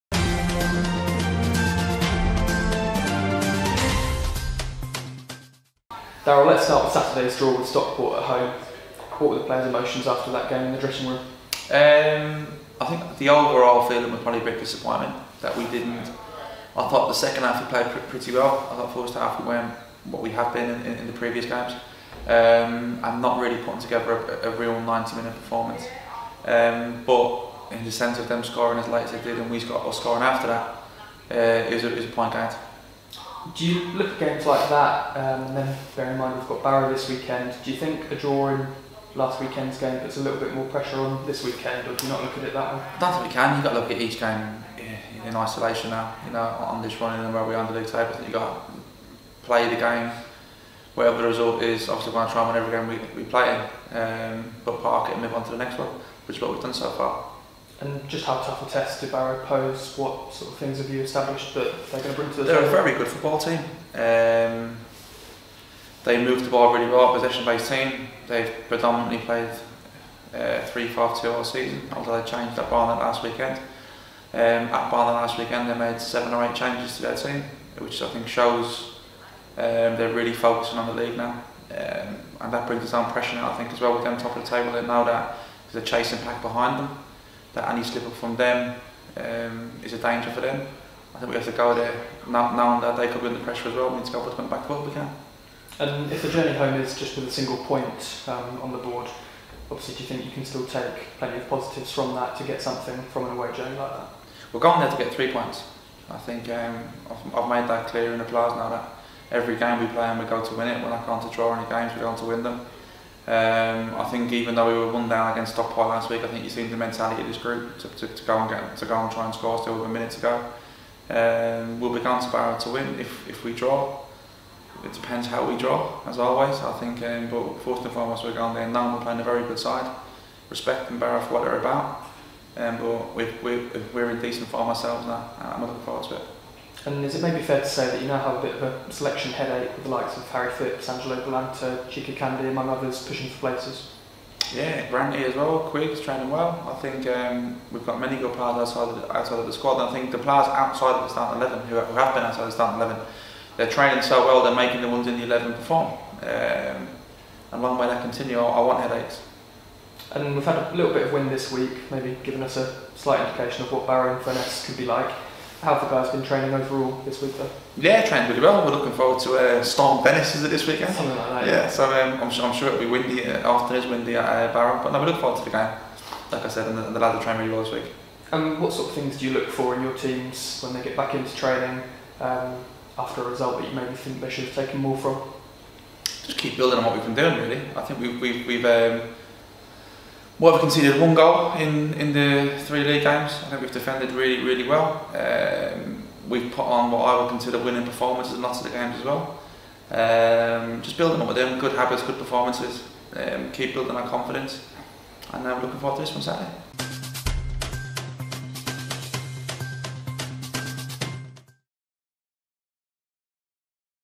INTERVIEW: Daryl McMahon Ahead Of Barrow AFC
DaggersTV spoke to the gaffer at the training ground this afternoon, to get his thoughts from this week's training, and on the long trip to Barrow AFC.